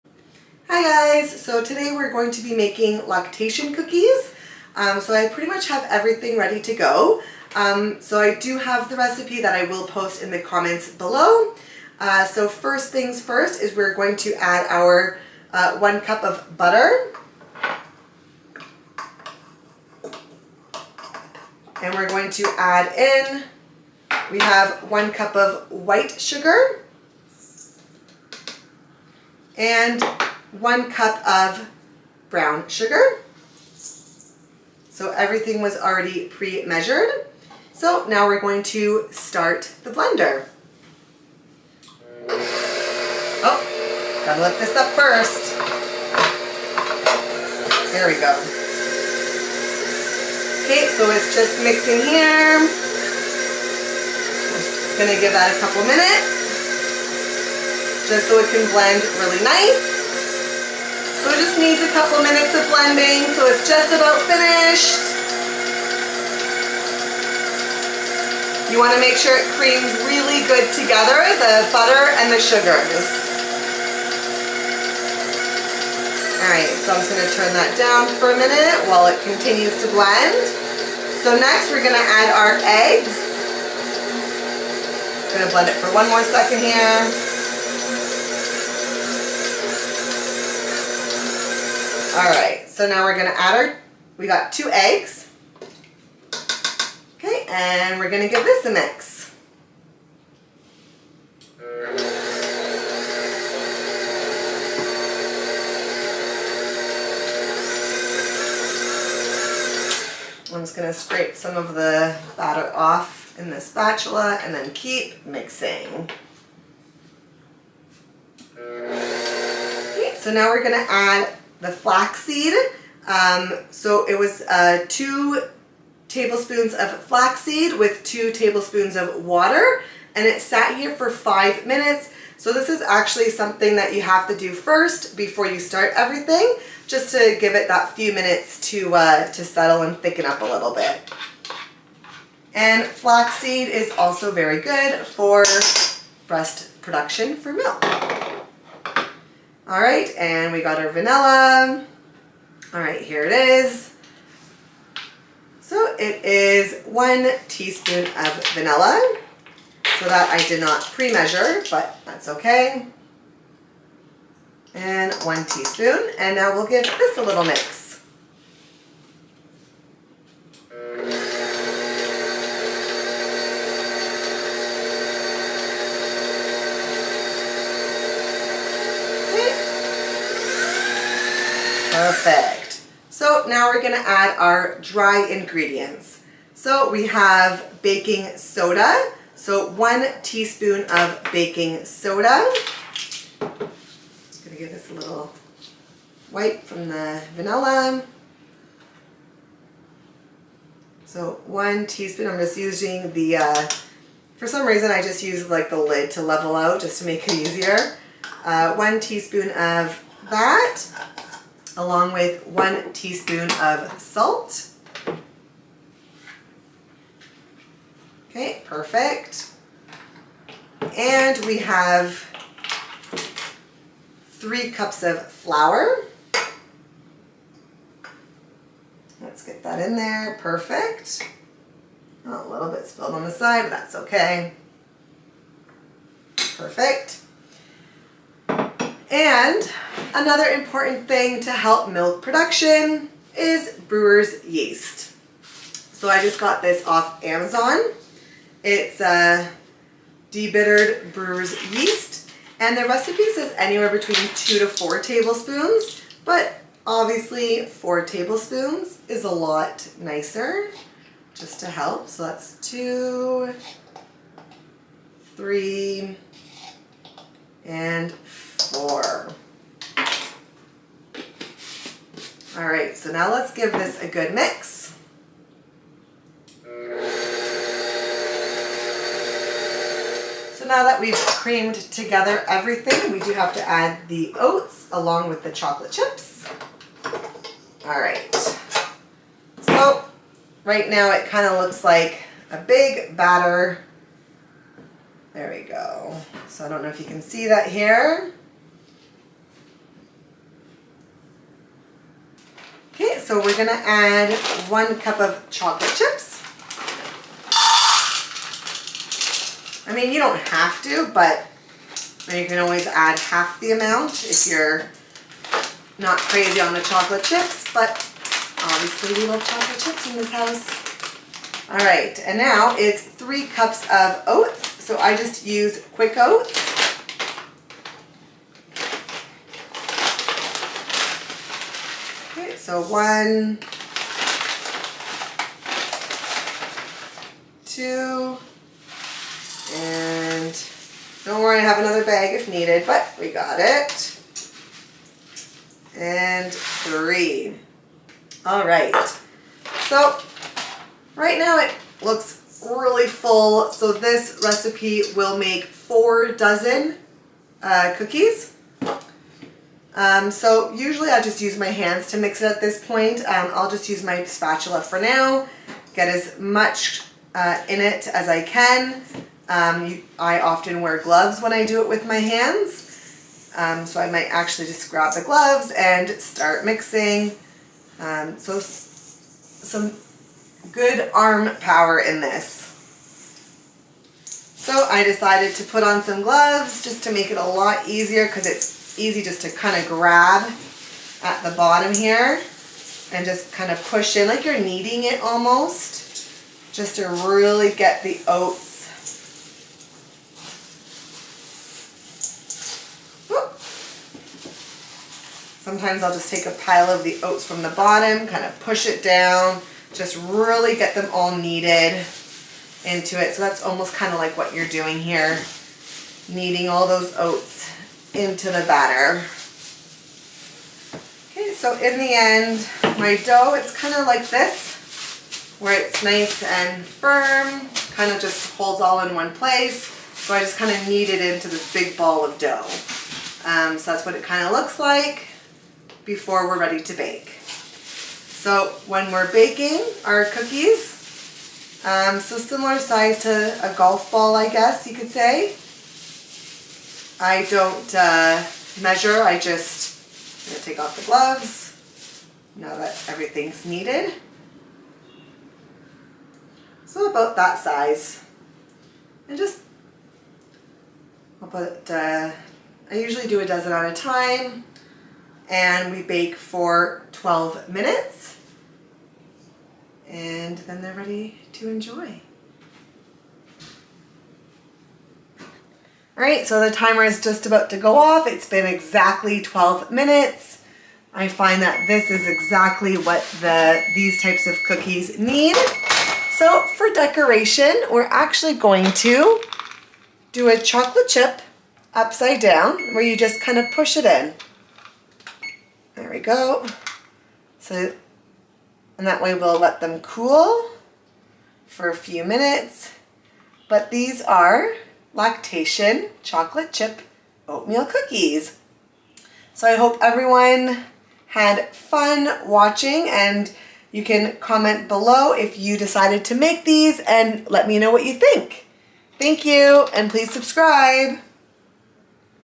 Lactation cookies - How to Make
These lactation cookies are super delicious and have had amazing reviews on these - both taste and with helping breast milk production.

Recipe:
1 cup butter
1 cup white sugar
1 cup brown sugar - packed
2 tbsp ground flaxseed mixed with 2 tbsp water
1 tsp vanilla
2 eggs
3 cups water
1 tsp baking soda
1 tsp salt
2-4 tbsp brewers yeast
3 cups oats
1 cup chocolate chips

Directions:
1) Mix flaxseed with water - sit for 3-5 minutes
2) cream butter and sugars
3) add egg and mix
4) add flaxseed mixture and vanilla and mix.
5) add dry (flour, brewers yeast, baking soda, salt) and mix
6) sit in oats and chocolate chips

Bake 12 minutes. Cool and enjoy!!!

What does Flaxseed do for milk production: flaxseed has phytoestrogens that can promote breast milk production.

What does oats do for milk production: Oatmeal is a good source of iron and high iron dosage can help with milk production.

What does brewers yeast for milk production: Brewer's yeast is full of vitamins and minerals that support breastfeeding. These nutrients include B vitamins and minerals like selenium and chromium.